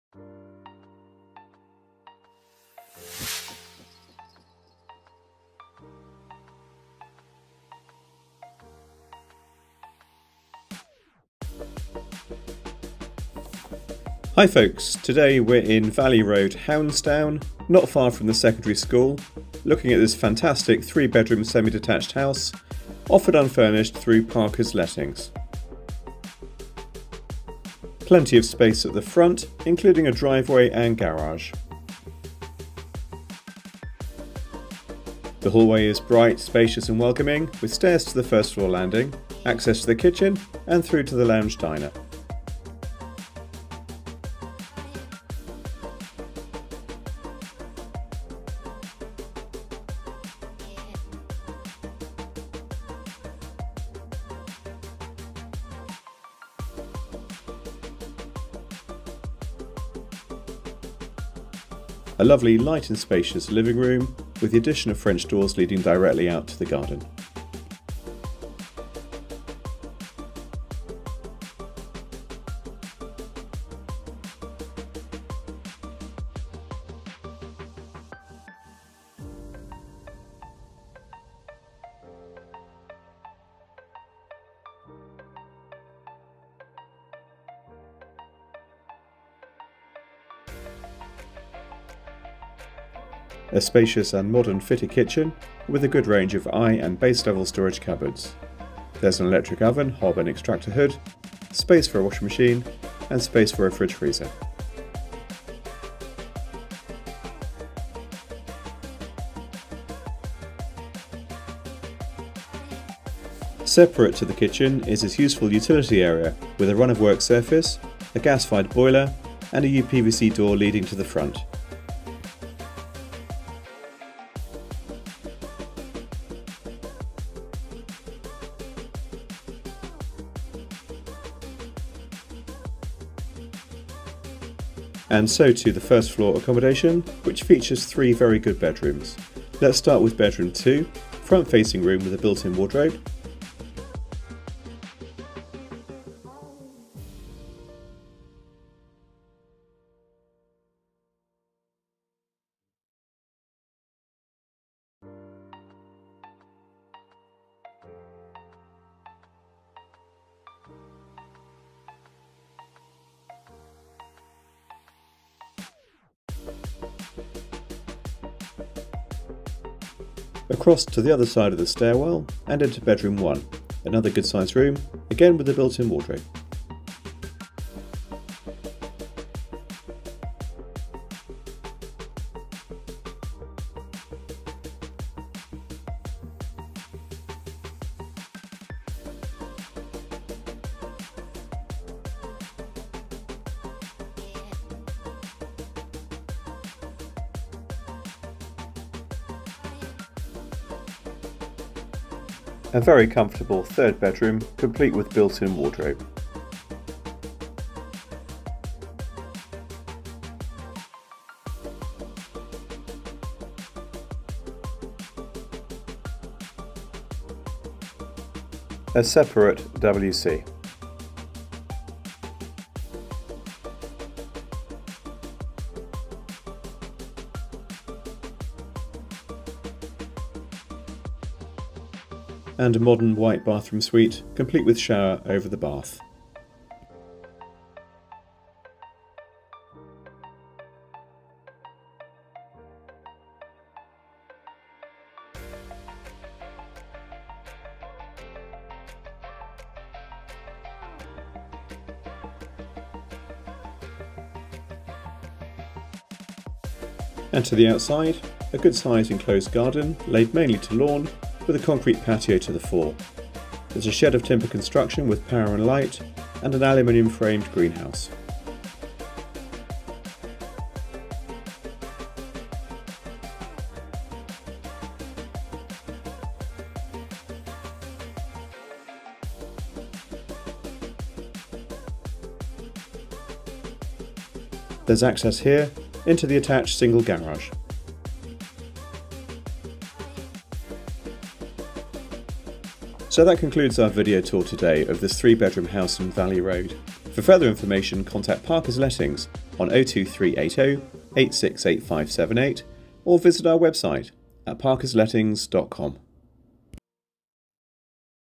Valley Road, Hounsdown - A Walk thru.
A beautifully-presented 3-bedroom semi-detached family house in this most convenient location, within a short walk of Hounsdown School.